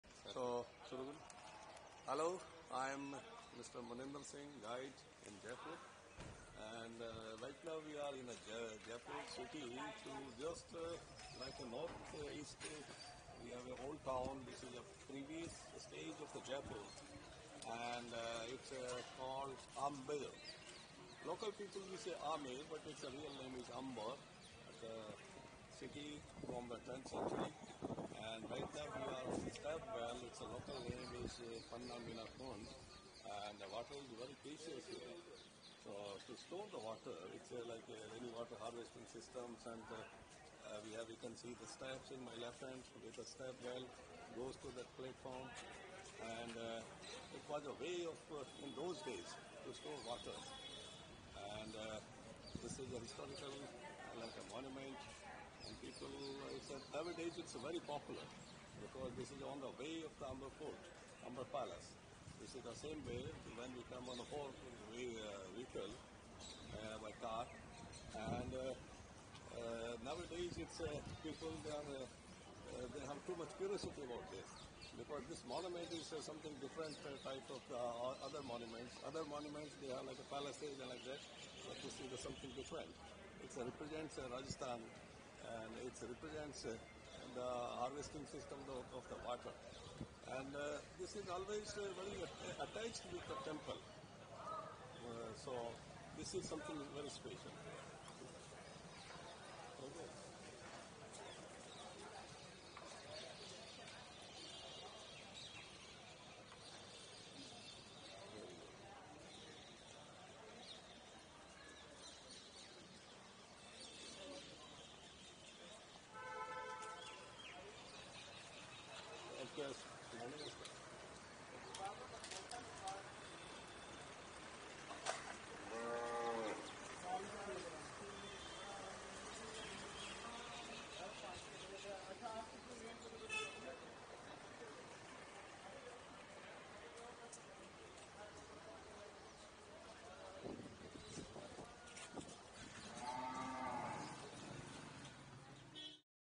PANNA MEENA KA KUND, JAIPUR IN 360
Mysterious steps well in near Amber Fort - Popular for photoshoot.